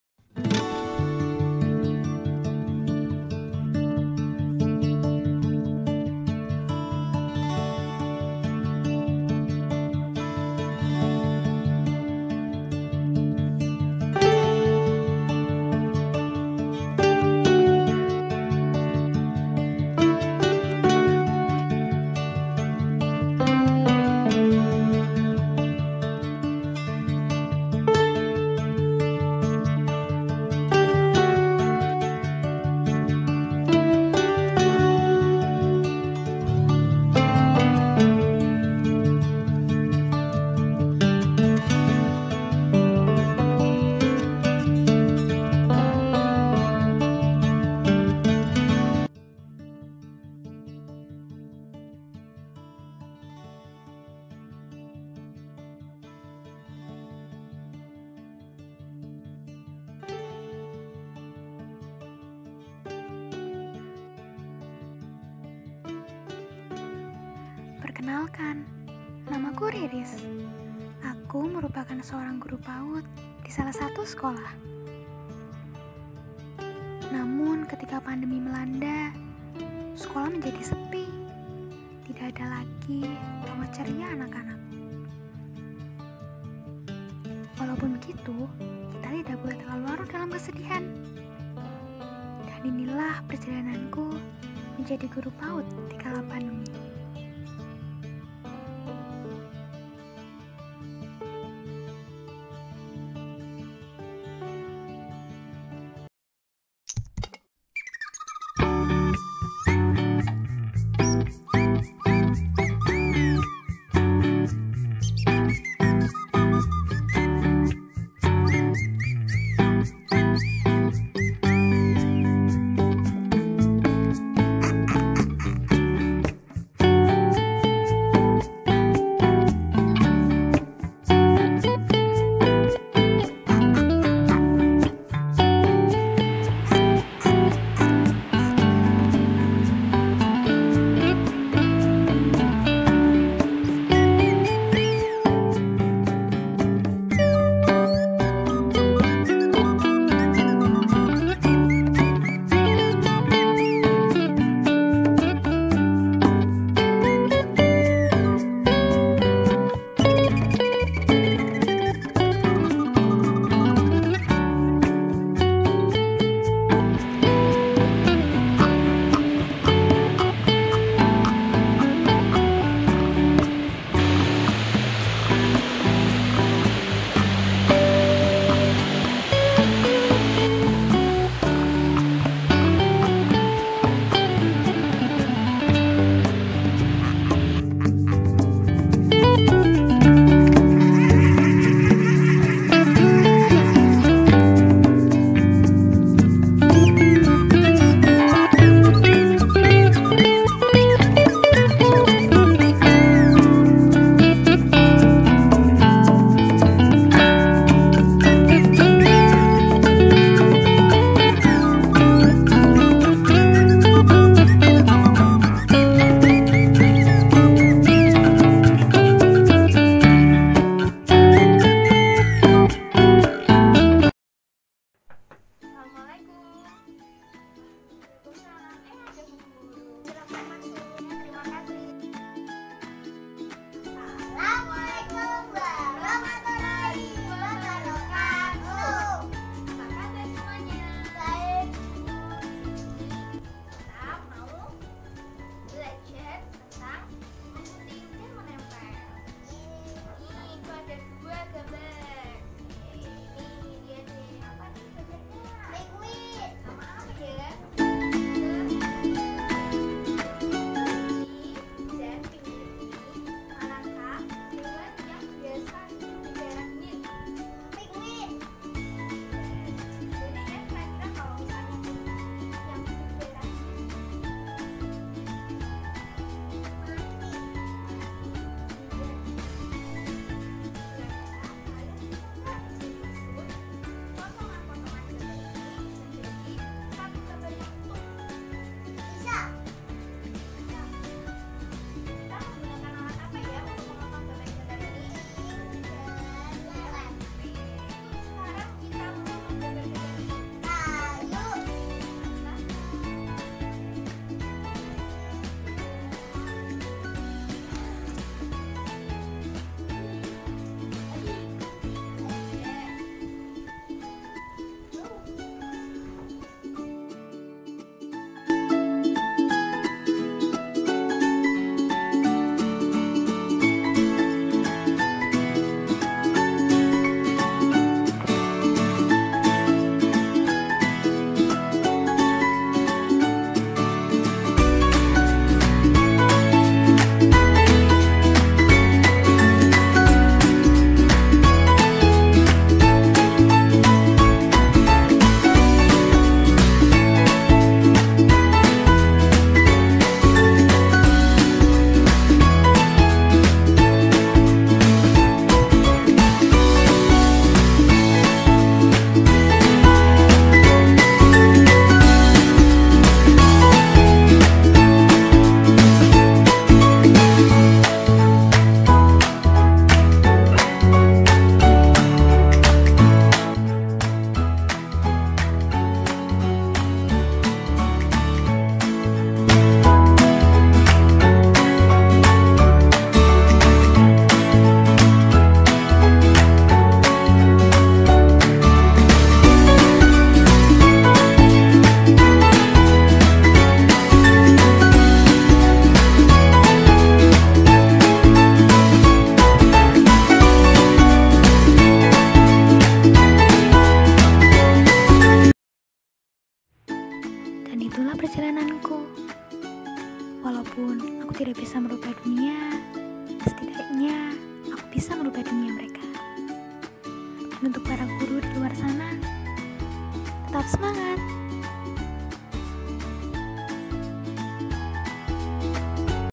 PERJALANAN - Di Balik Layar Menjadi Guru PAUD di Kala Pandemi -
Assalamualaikum semua 🌻
Video ini dibuat untuk memenuhi tugas Mata Kuliah Sinematografi Semester 5
Nama : Rizqi Nurzaini Putri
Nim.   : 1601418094

Video ini menceritakan tentang perjalanan salah satu guru PAUD yang melakukan kegiatan home visit di salah satu rumah anak didiknya untuk berbagi keceriaan.

Pesan pada video ini adalah agar para guru di luar sana tetap semangat dalam mengajar anak didiknya walaupun sedang dalam keadaan pandemi seperti saat ini.

Mohon maaf atas segala kekurangan yang terdapat pada video ini dan semoga kedepannya akan lebih baik lagi.

Terima kasih.
Wassalamu'alaikum 🌻